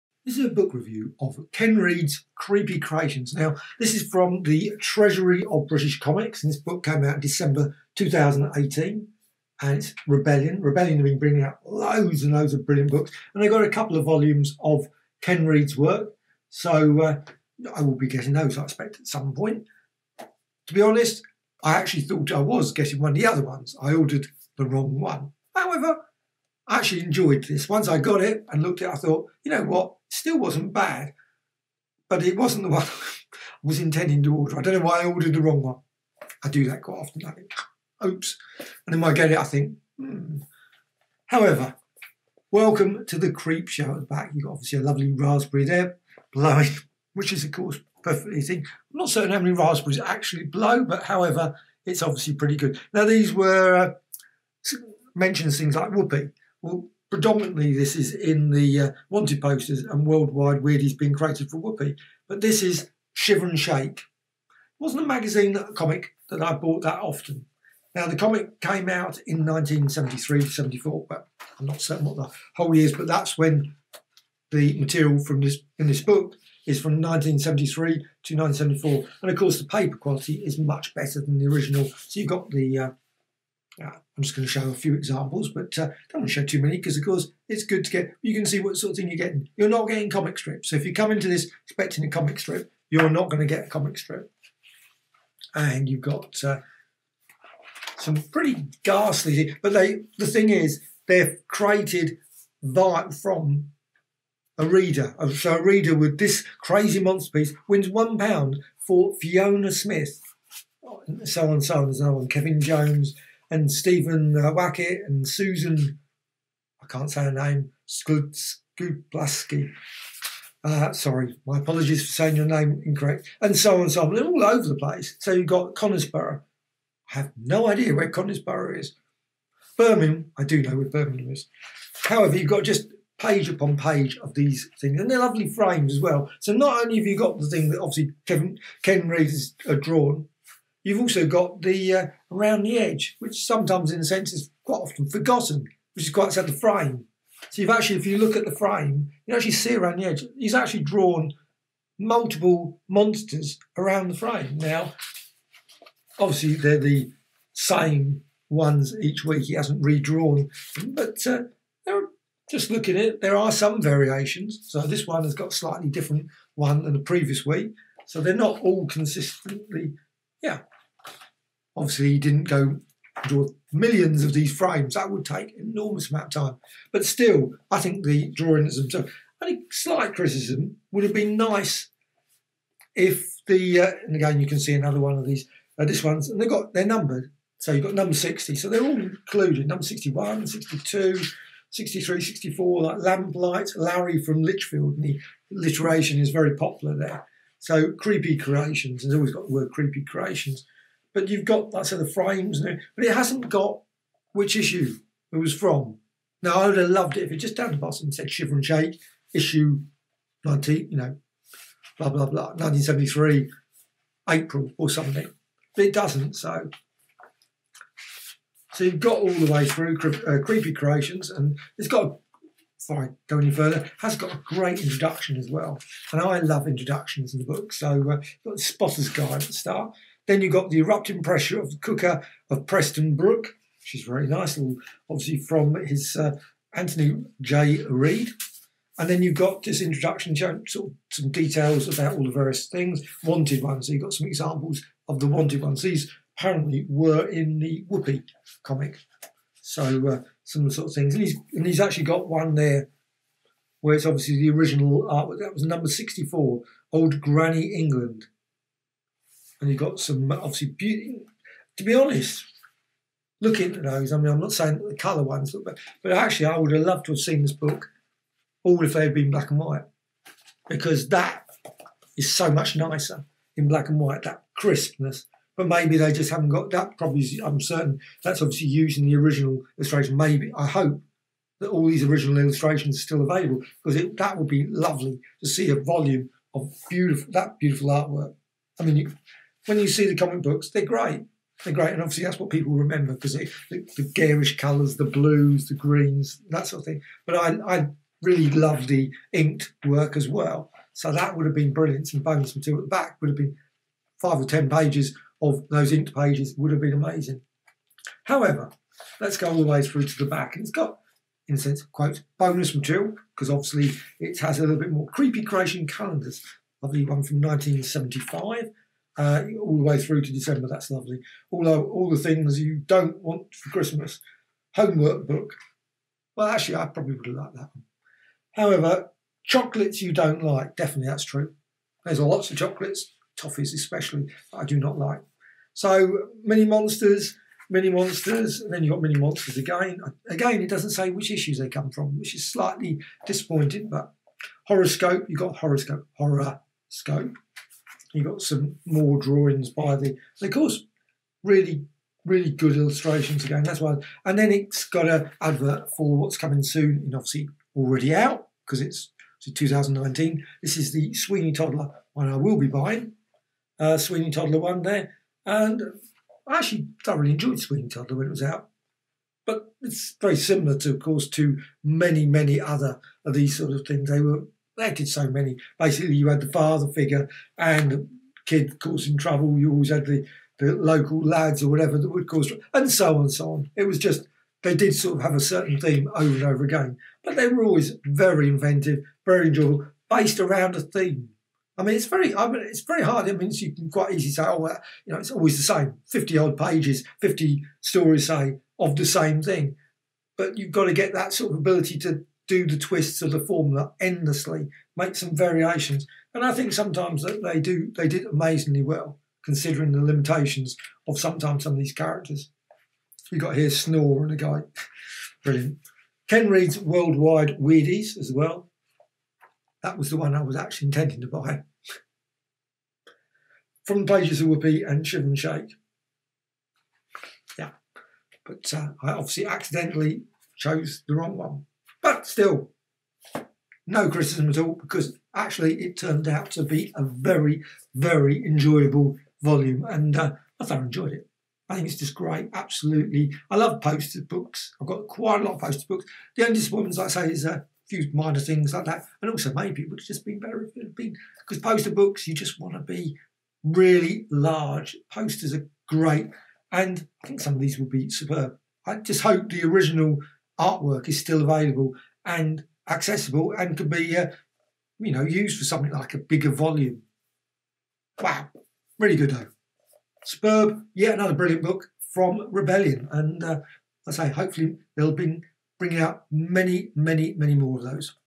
Ken Reid's Creepy Creations from Rebellion UK Comics (Shiver and Shake) Book Review
Ken Reid's Creepy Creations from Rebellion UK Comics (Shiver n Shake) Book Review - a superb collection of all the Creepy Creations drawn by Ken Reid from the reader's suggestions along with an intro and details about the posters / creations. From Rebellion and part of their excellent Treasury of British Comics